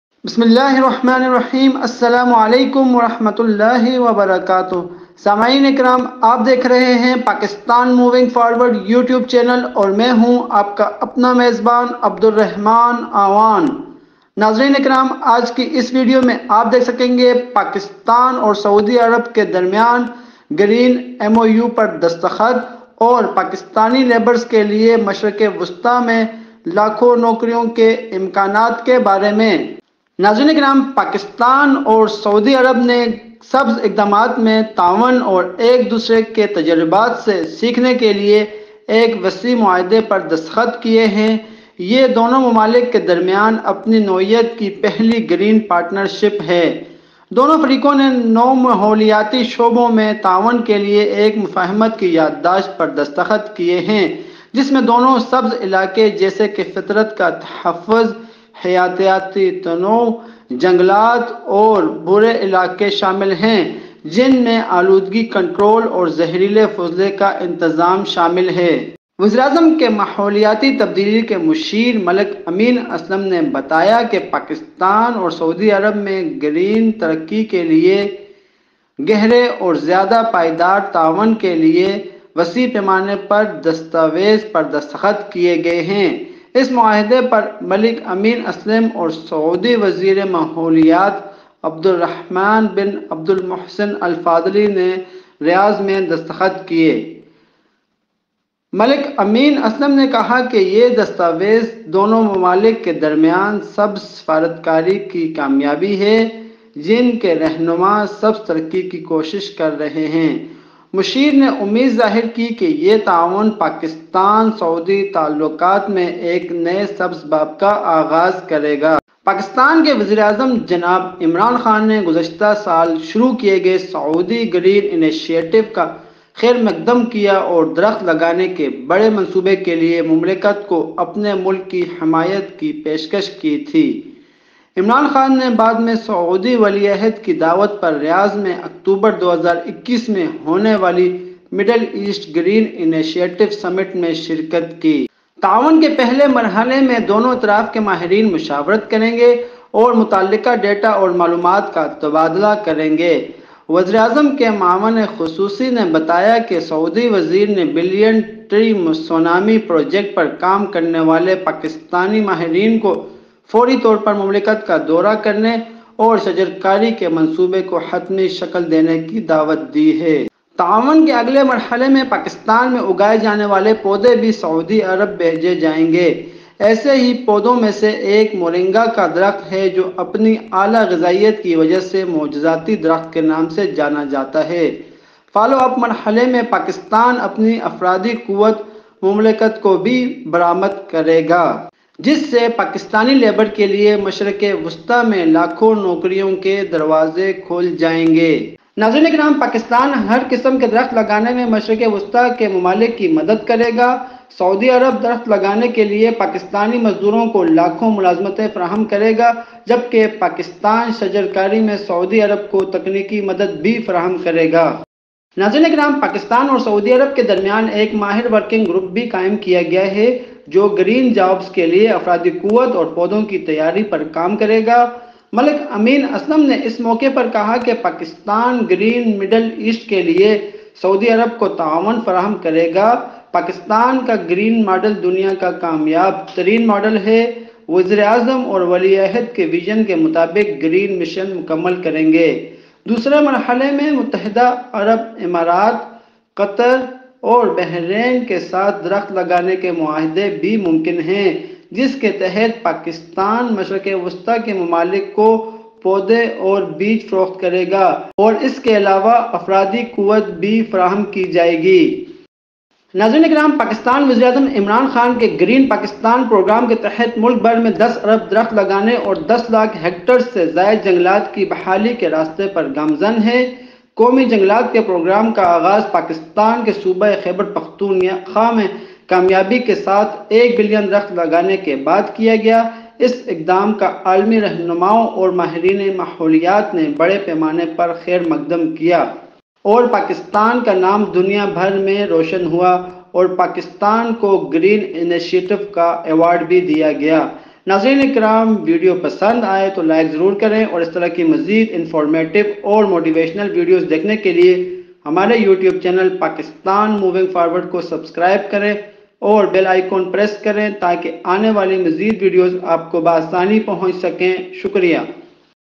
Agreement Signed To Create Green Jobs For Pakistanis In Middle East (KSA -Pakistan green Initiative)
The door has been opened for Pakistani labor to get millions of jobs in the Middle East. A Green MoU has been signed between Pakistan and Saudi Arabia under the Mission Green Middle East of Prime Minister Imran Khan and Saudi Crown Prince Muhammad bin Salman.
According to details, Pakistan has partnered with Saudi Arabia to plant billions of trees in the Middle East, under which Pakistani labor will get green jobs in the Middle East.
Pakistan will assist Middle Eastern countries in planting all kinds of trees. Saudi Arabia will provide millions of jobs to Pakistani labor for tree planting, while Pakistan will also provide technical assistance to Saudi Arabia in plantation.
In the second phase, tree planting agreements with the United Arab Emirates, Qatar and Bahrain are also possible, under which Pakistan will sell plants and seeds to Middle Eastern countries.